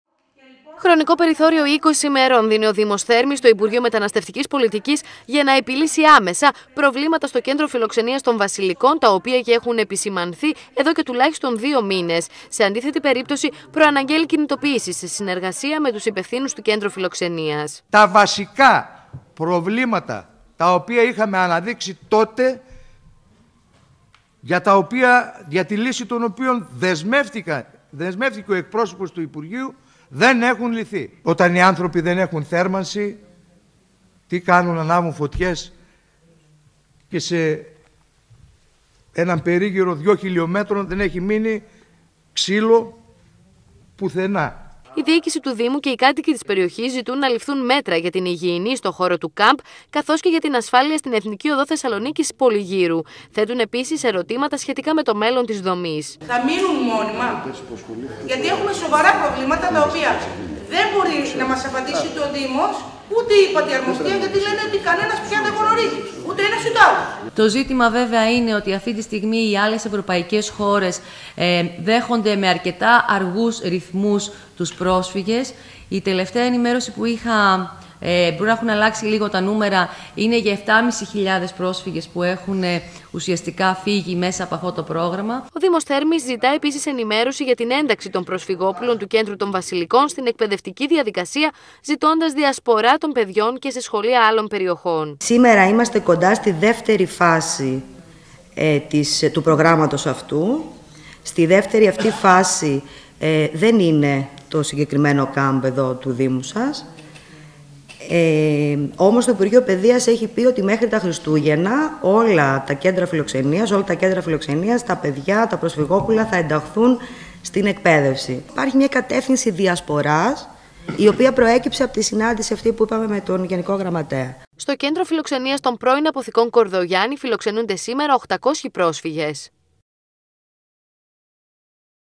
ΘΕΡΜΗ-ΚΕΝΤΡΟ ΦΙΛΟΞΕΝΙΑΣ ΠΡΟΣΦΥΓΩΝ-ΔΗΜΟΤΙΚΟ ΣΥΜΒΟΥΛΙΟ(TV100-101116)
Τελεσίγραφο μέχρι το τέλος του μήνα δίνει στην κυβέρνηση και στο αρμόδιο υπουργείο Μεταναστευτικής Πολιτικής ο δήμος Θέρμης, ώστε να προχωρήσουν στην επίλυση προβλημάτων που έχουν επισημανθεί στη λειτουργία του Κέντρου Προσφύγων στην περιοχή των Βασιλικών. Σε αντίθετη περίπτωση, η διοίκηση του δήμου προαναγγέλλει δυναμικές κινητοποιήσεις, σε συνεργασία μάλιστα με τους υπευθύνους λειτουργίας του Camp. Στη χθεσινή συνεδρίαση του Δημοτικού Συμβουλίου Θέρμης επισημάνθηκαν προβλήματα που αφορούν στη θέρμανση του χώρου, τις συνθήκες υγιεινής, καθώς και στη φύλαξη της Εθνικής Οδού Θεσσαλονίκης-Πολυγύρου.
Ρεπορτάζ: Βάσω Λυκουρίνου
Κάμερα: Γιώργος Βογιατζόγλου
Μιλούν: ΘΟΔΩΡΟΣ ΠΑΠΑΔΟΠΟΥΛΟΣ(Δήμαρχος Θέρμης),
ΕΛΕΑΝΝΑ ΙΩΑΝΝΙΔΟΥ(Συντ. Εργου Υπατης Αρμοστείας ΟΗΕ για τους Πρόσφυγες Β.Ε.),
ΦΩΤΕΙΝΗ ΠΑΠΑΔΟΠΟΥΛΟΥ(Εκπρ. Περ. Δ/νσης Εκπαίδευσης Κ. Μακεδονίας)